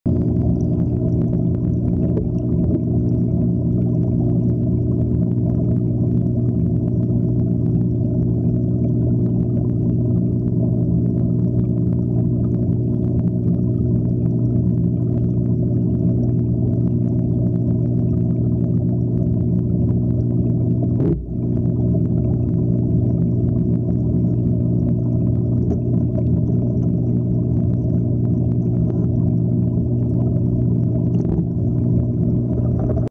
Sebae Anemone & Yellow Tip Hermit Crab
Tank Marine Life at Aquarium Illusions, even the little hermit crabs are interesting and play a role in the ecosystem of a coral reef aquarium